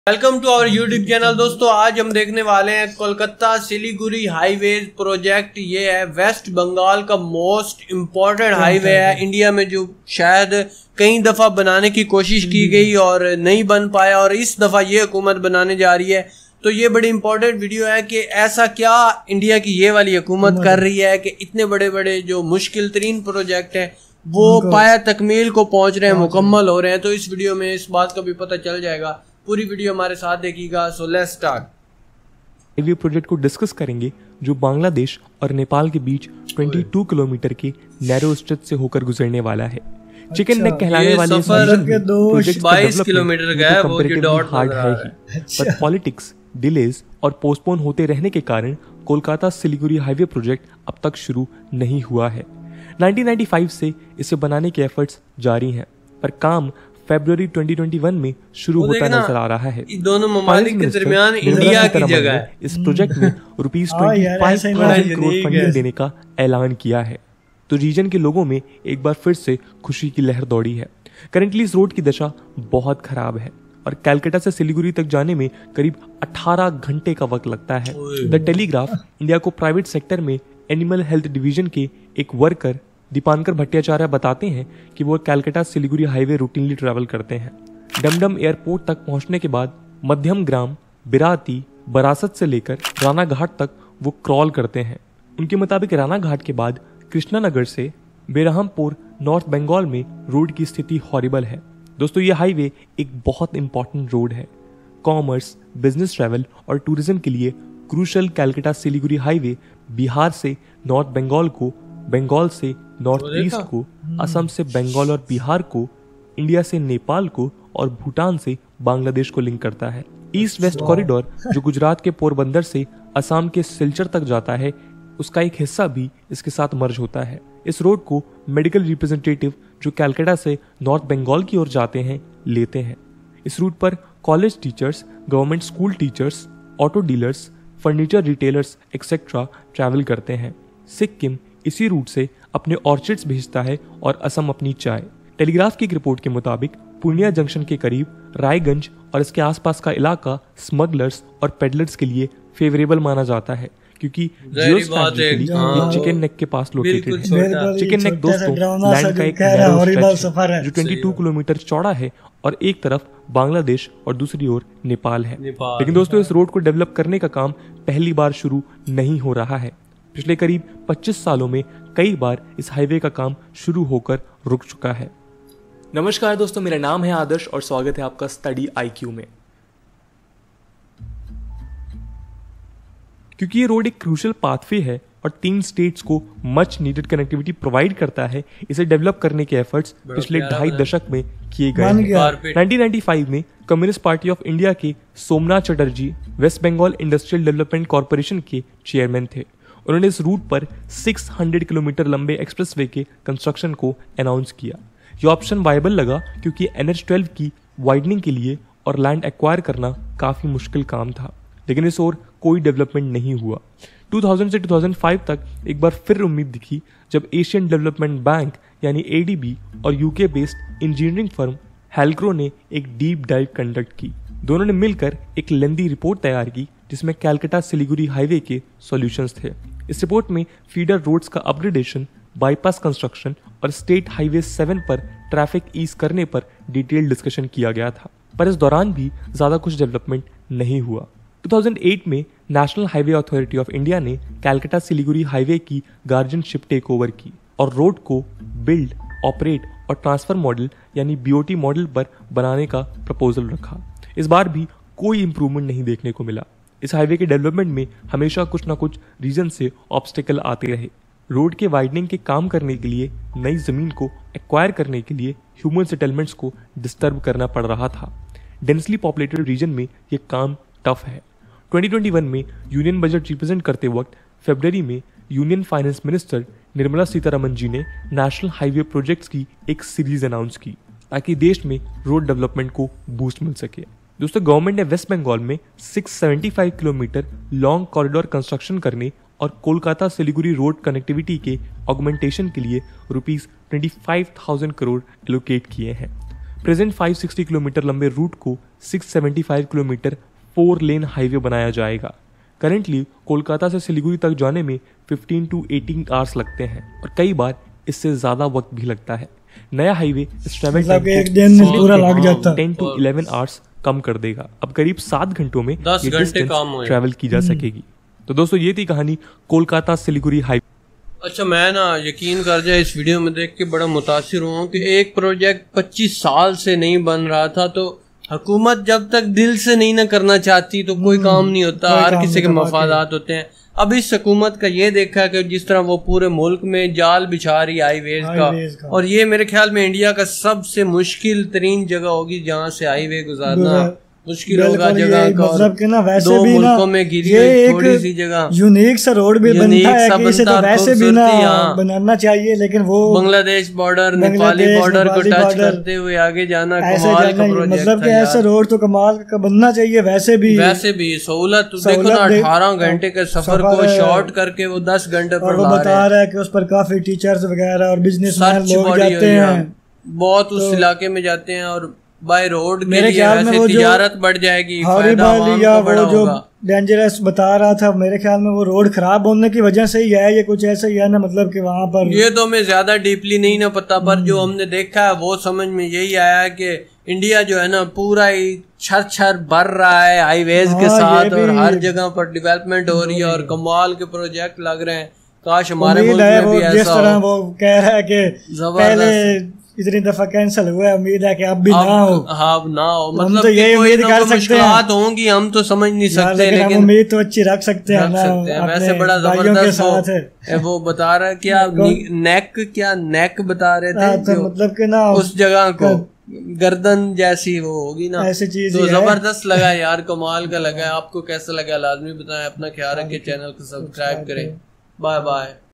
Pakistani reaction on Kolkata Siliguri Highway Project - West Bengal's most important highway 2021
Courtesy -  Study IQ education